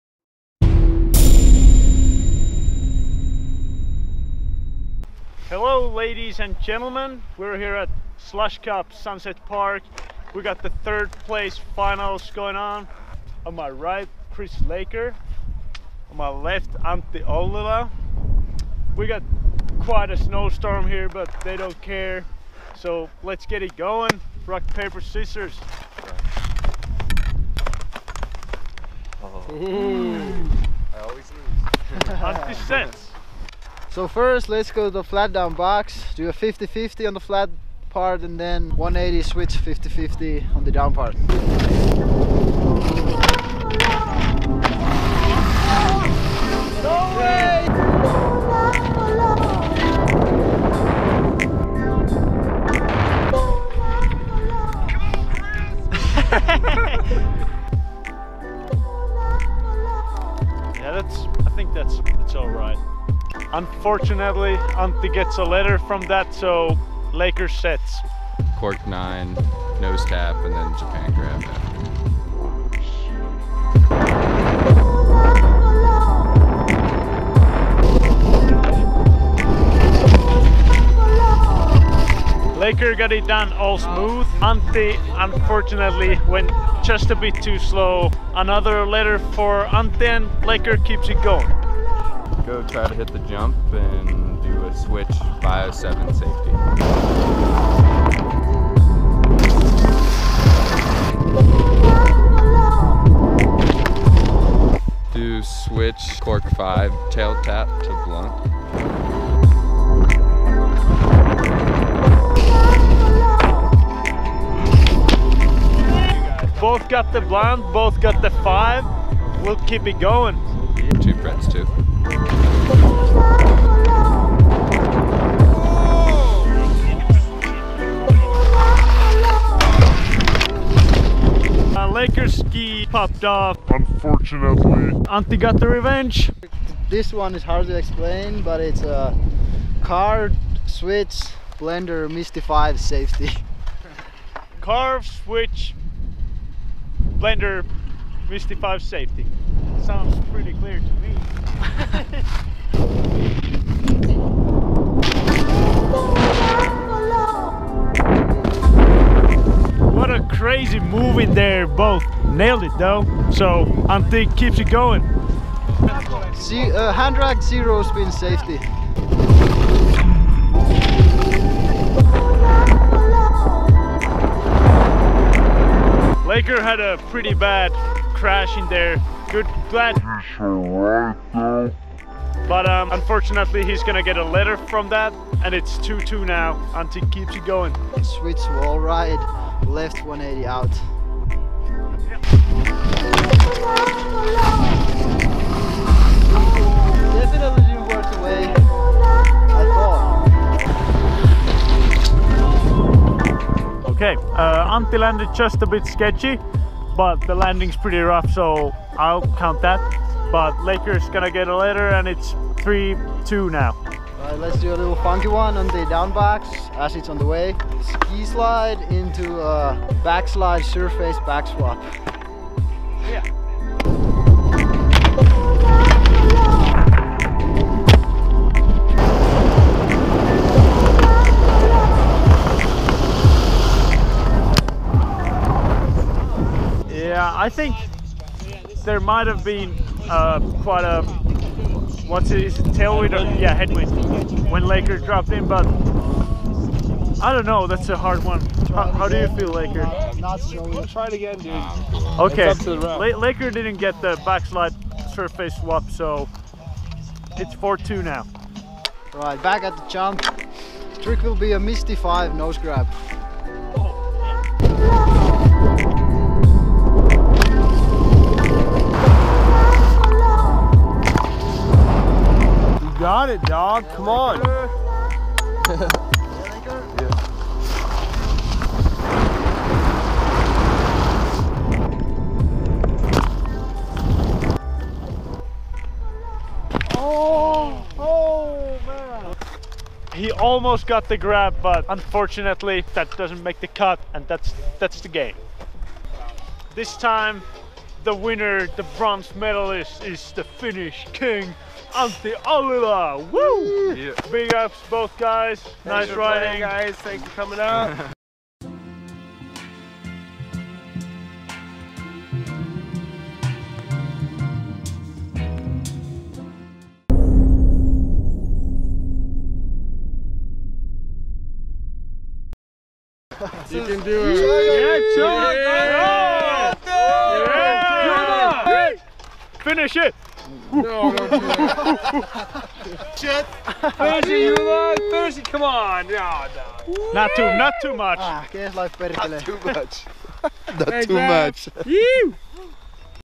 15. SLVSH CUP SUNSET PARK CONSOLATION 2016 || Antti Olilla vs. Chris Laker
Antti Olilla and Chris Laker face off in the Consolation Finals of the 2016 SLVSH CUP SUNSET PARK at Grandvalira in Andorra || For new games dropping every Tuesday,